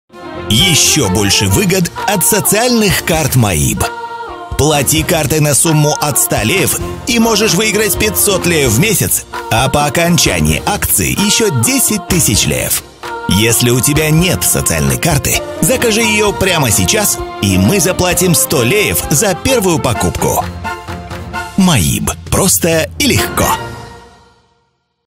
«Еще больше выгод от социальных карт maib!»
Плати своей социальной картой на сумму от 100 леев и участвуй в акции!
У тебя есть шанс выиграть 500 леев в месяц, а по окончании акции - еще 10 000 леев.
Если у тебя нет социальной карты, закажи ее прямо сейчас, и мы заплатим 100 леев за первую покупку.

Период акции: 16 мая – 31 августа 2022 г.

Maib - просто и легко!